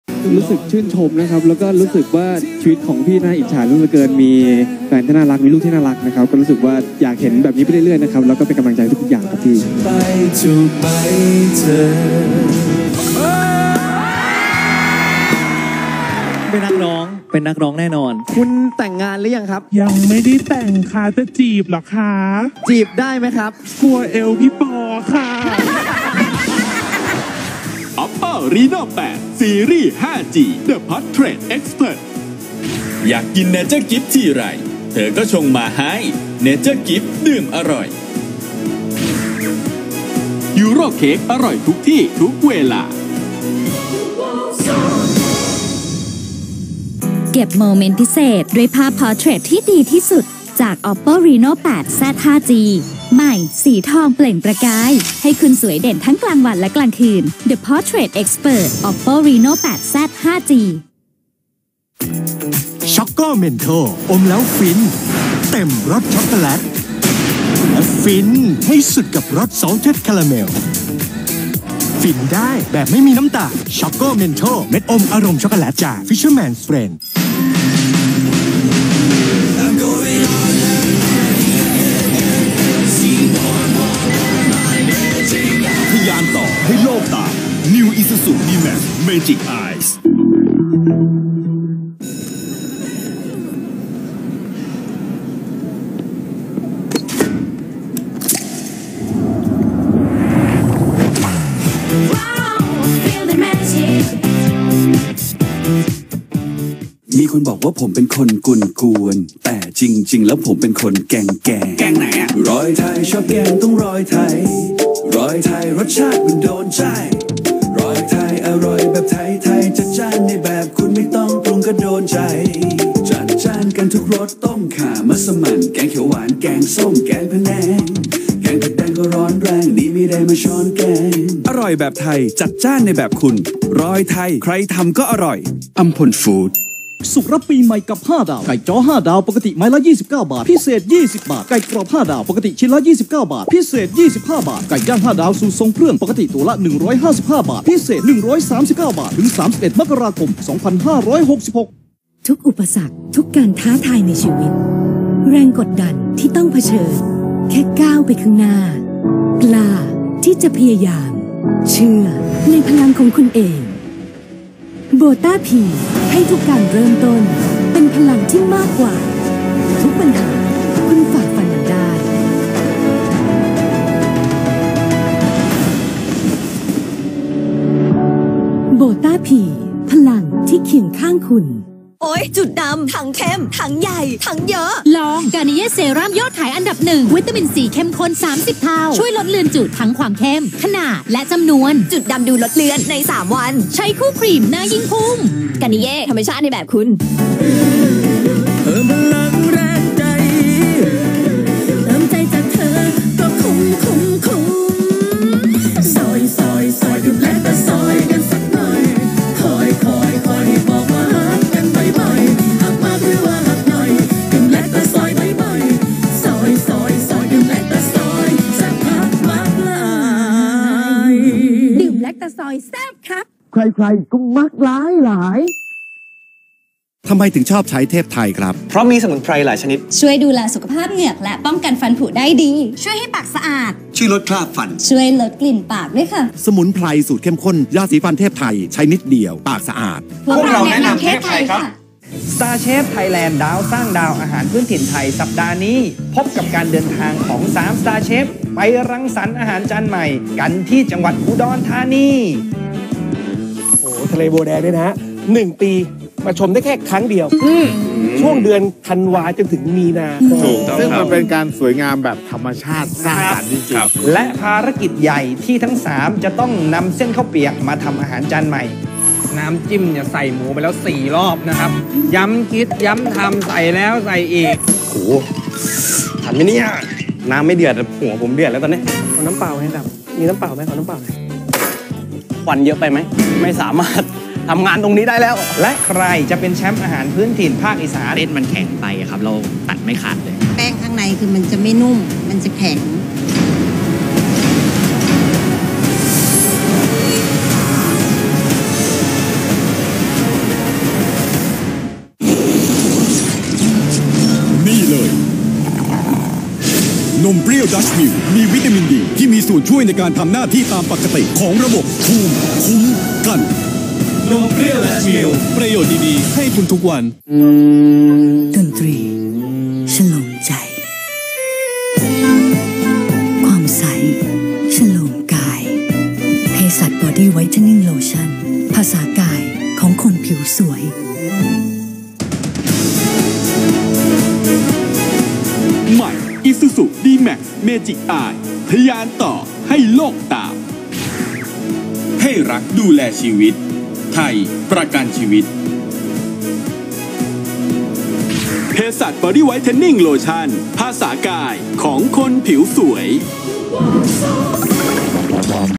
โฆษณาไทย 2566 - 30
โฆษณาคั่น ในรายการ ร้องข้ามกำแพง ทางช่อง workpoint23 ในปีพ.ศ. 2566